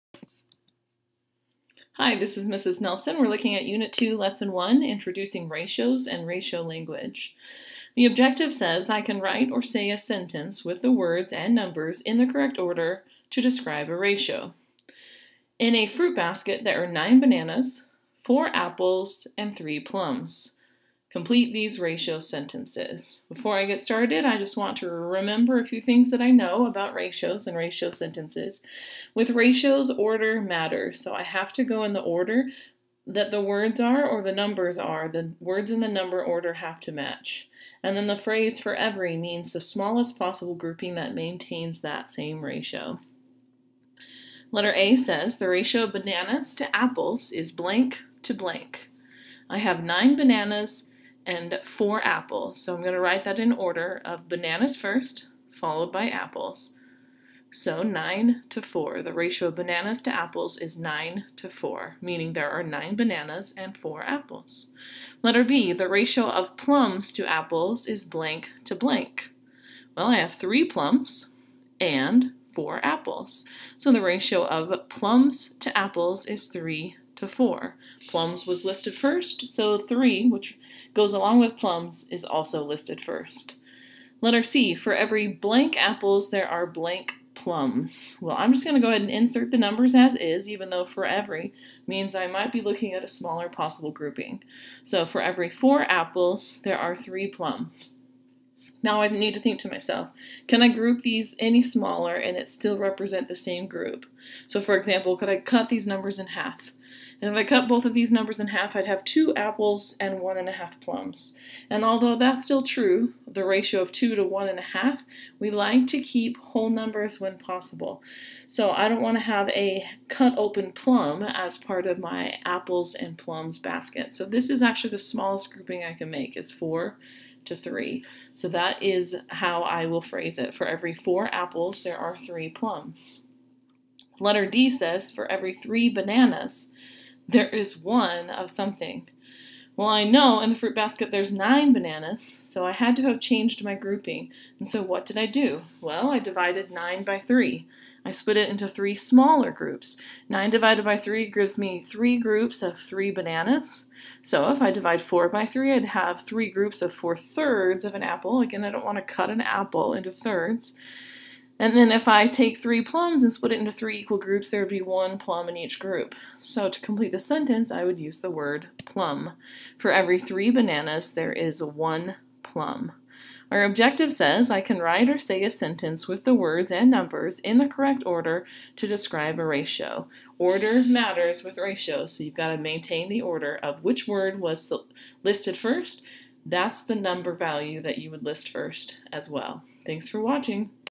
Introducing Ratios and Ratio Language Grade 6 Unit 2 Lesson 1
Objective: I can write or say a sentence with the words and numbers in the correct order to describe a ratio.